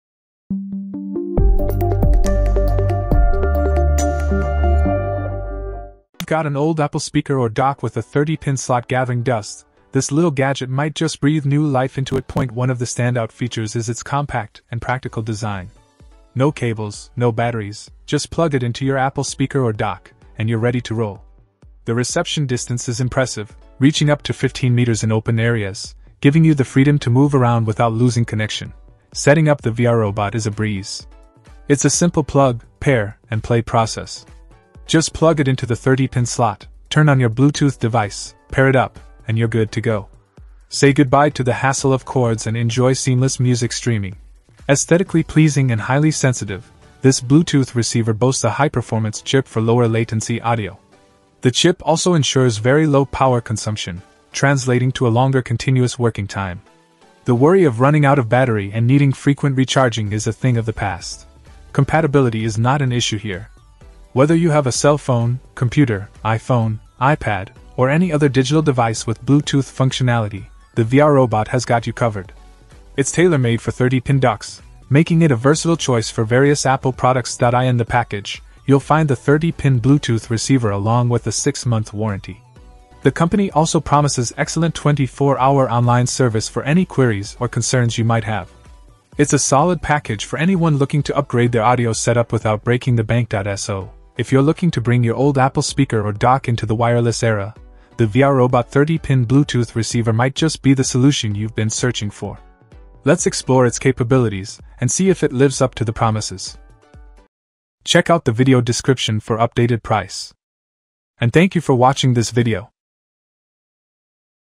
Transform Your Wired Speakers with VR-robot 30 Pin Bluetooth Receiver | Honest Review
0:00 Intro
0:06 Review

Things that we mentioned in this video:
VR robot 30 Pin : B0CH9YPYHJ